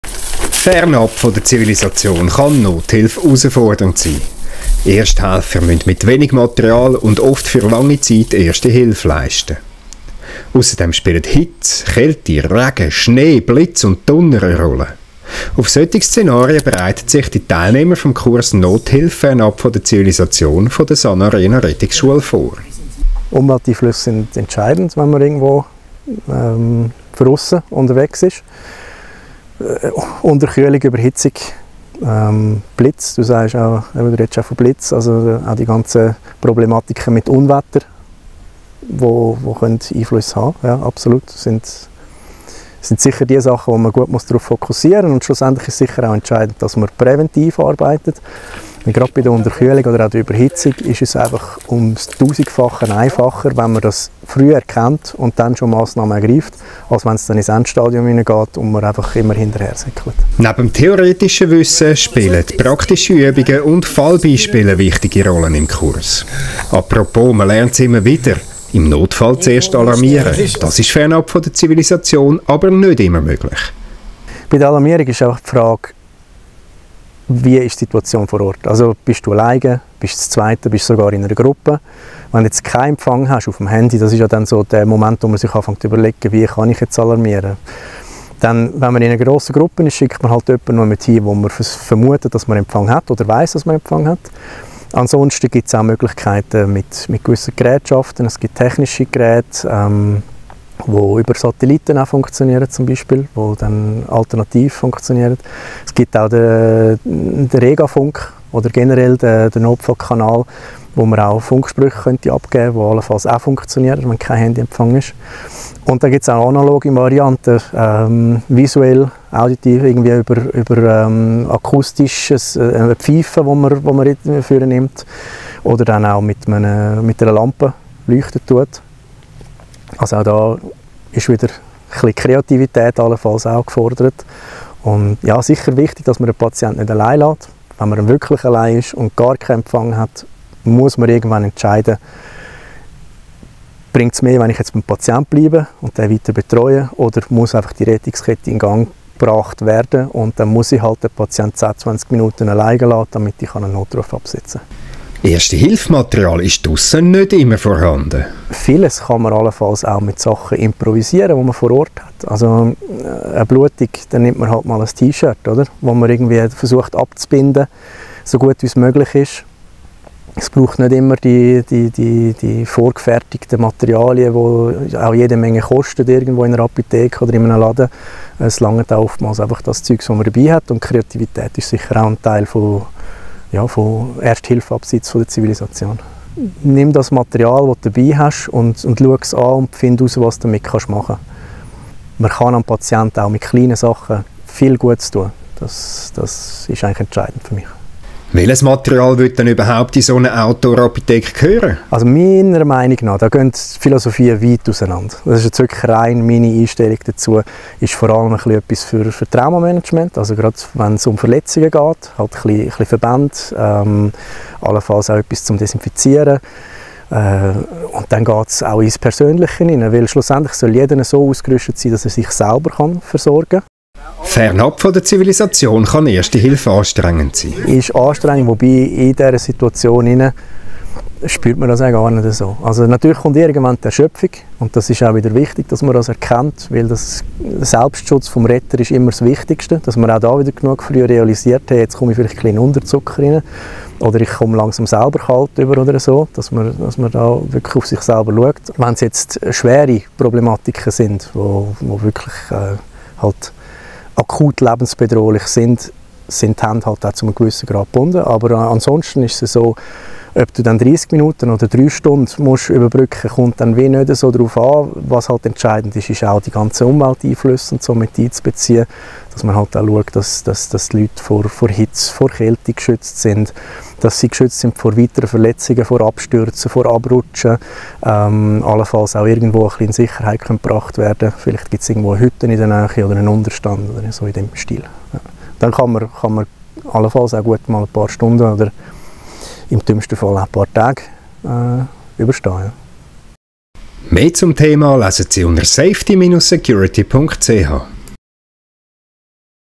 Erste Hilfe fernab der Zivilisation: Kreativität, Ausdauer und Selbstschutz sind gefordert
Biken, joggen, wandern, spazieren, langlaufen – draussen sein tut gut, ausser es passiert etwas. Dann stehen auch Ersthelfer:innen vor Herausforderungen. Ihnen fehlt Erste-Hilfe-Material, die Umweltbedingungen sind garstig, sie suchen ein Handy oder Handy-Empfang, um zu alarmieren, und es kann sehr lange dauern, bis zusätzliche Hilfe vor Ort ist. Der Kurs «Erste Hilfe fernab der Zivilisation» der SanArena Rettungsschule nahm sich diesen Herausforderungen an.